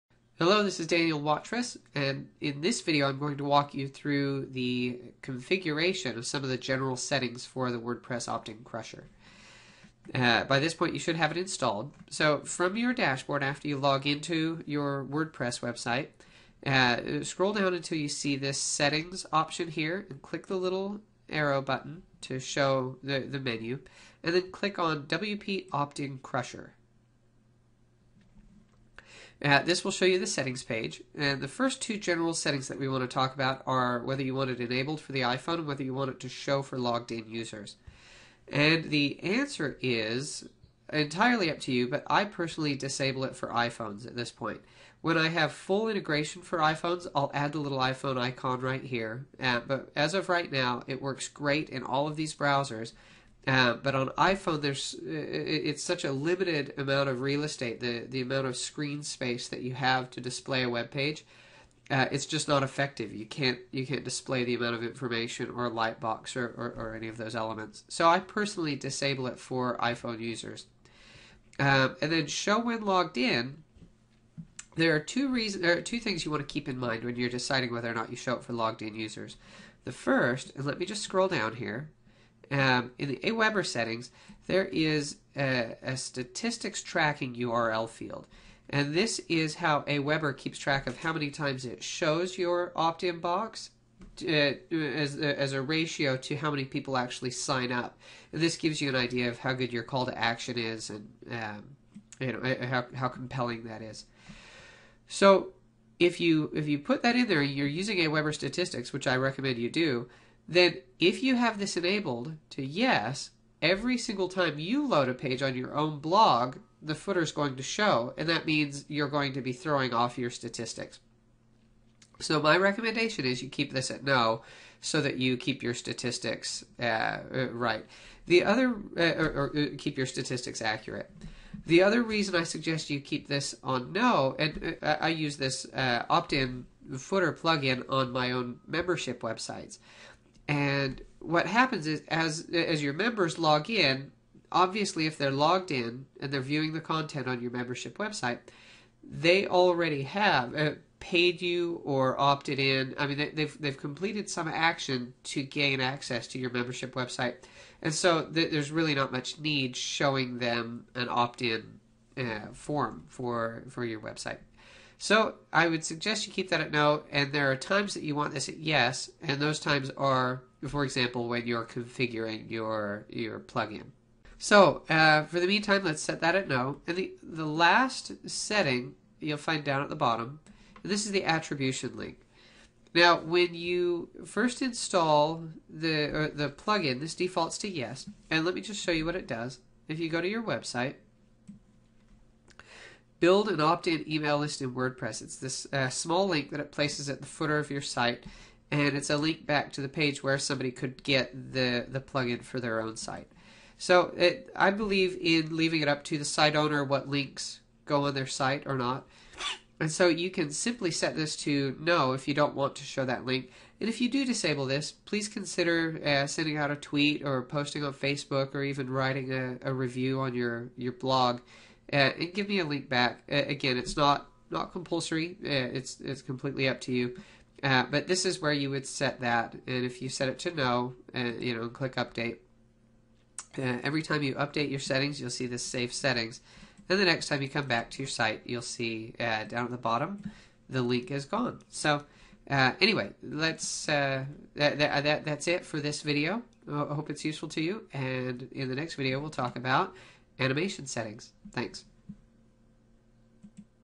WordPress Optin Crusher General Settings
This video walks you through configuration of the general settings in WordPress Optin Crusher. The optin crusher is free software that helps you build an optin email list in WordPress.